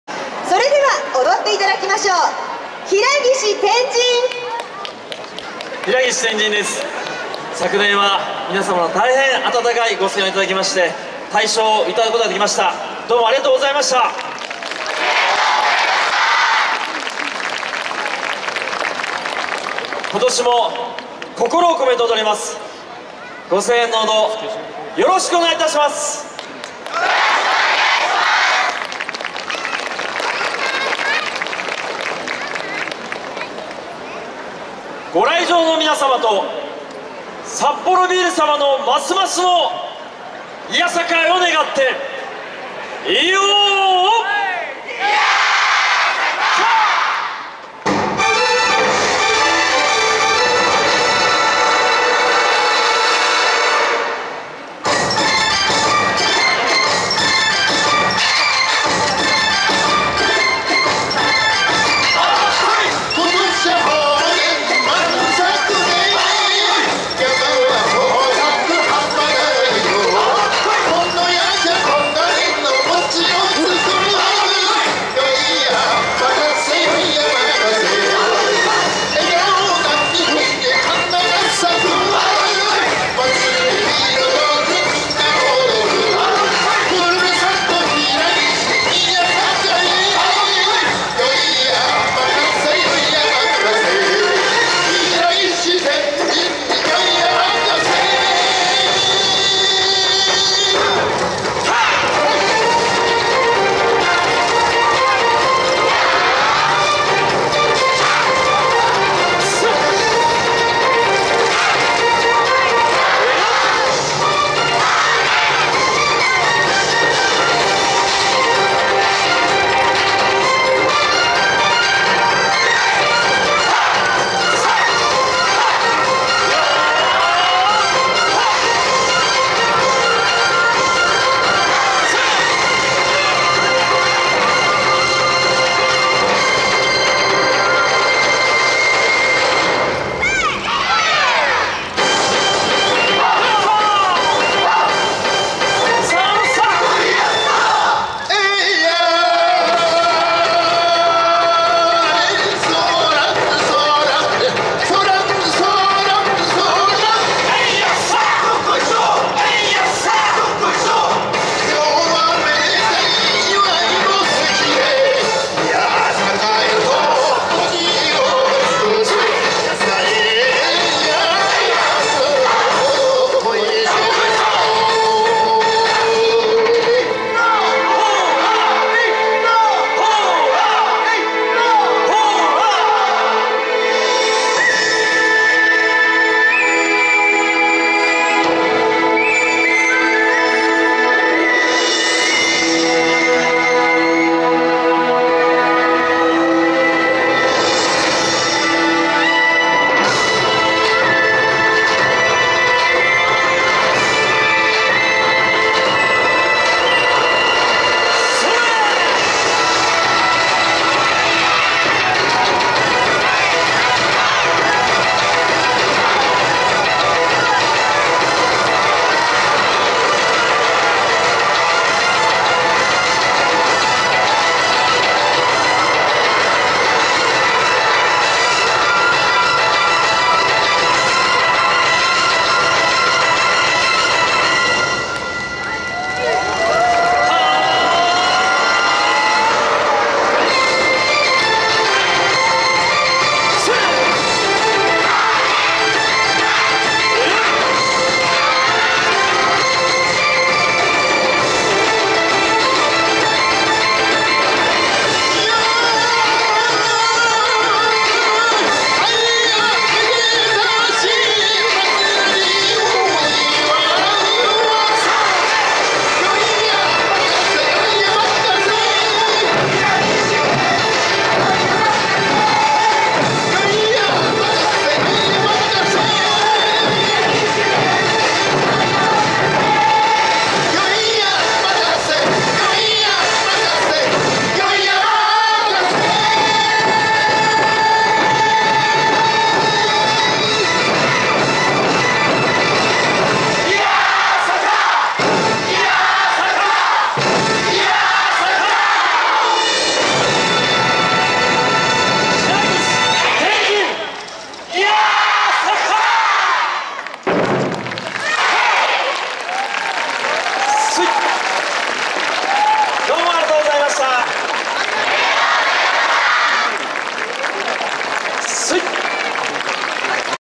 160611 第25回YOSAKOIソーラン祭り 平岸天神 ガーデンパーク
2016年6月11日、第25回YOSAKOIソーラン祭り ４日目。
ガーデンパーク（サッポロビール園）で行われた、平岸天神の演舞です。